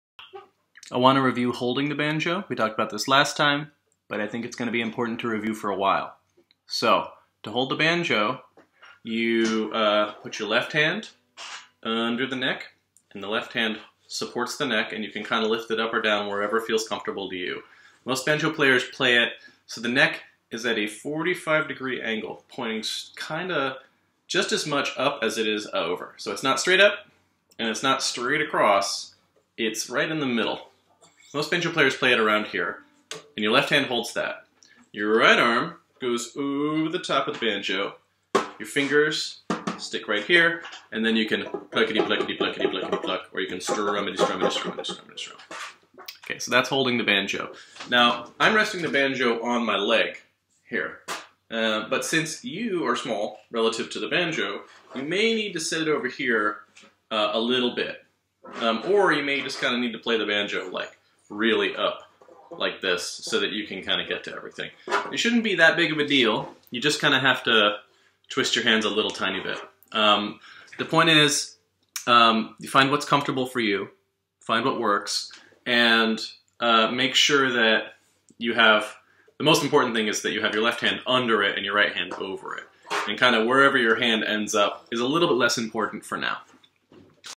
Banjo 3 - Holding the Banjo
How to hold the banjo in a way that works, given that you're probably smaller than the banjo when you're sitting down.